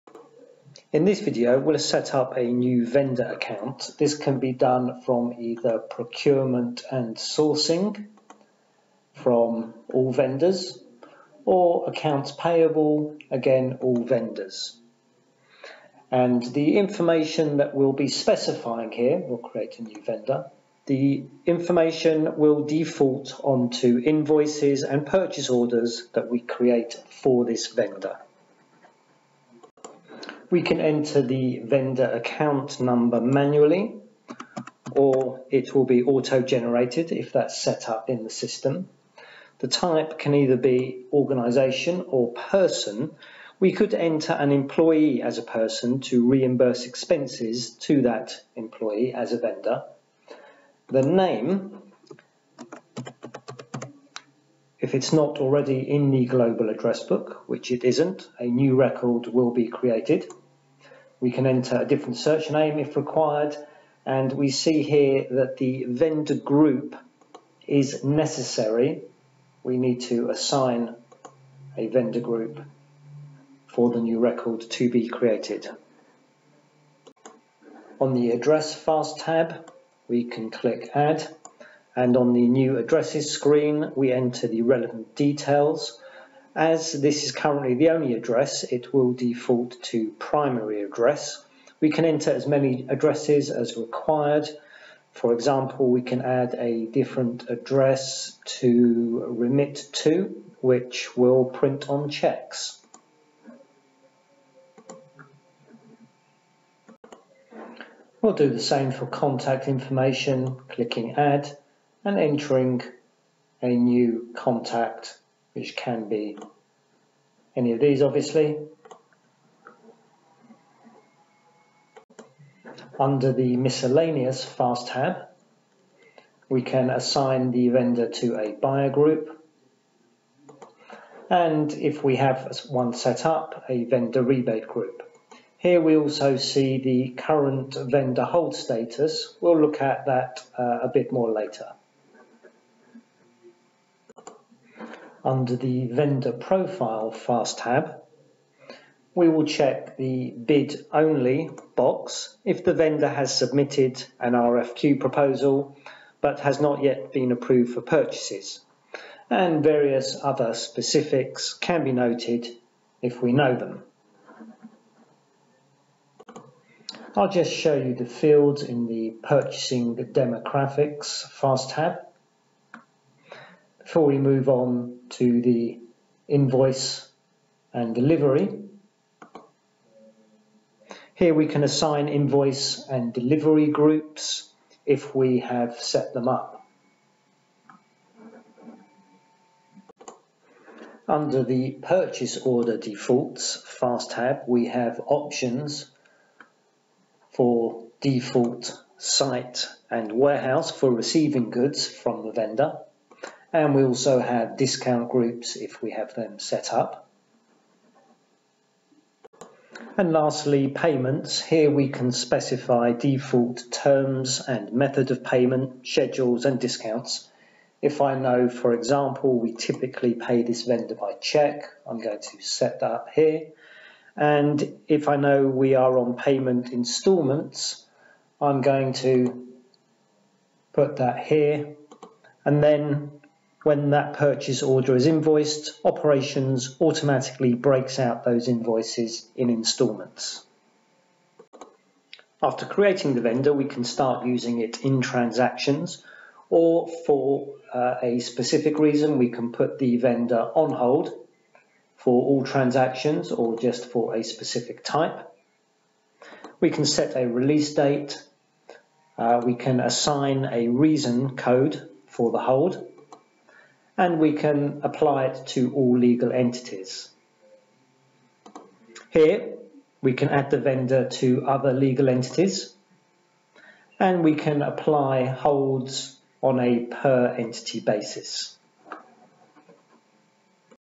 Microsoft Dynamics 365 for Finance and Operations: Create Vendor Accounts
This video shows how to create a new Vendor Account. Microsoft Dynamics 365 for Finance and Operations (formerly Dynamics AX) is a comprehensive enterprise resource planning (ERP) suite.
For more information about the capabilities of the software you can talk to a consultant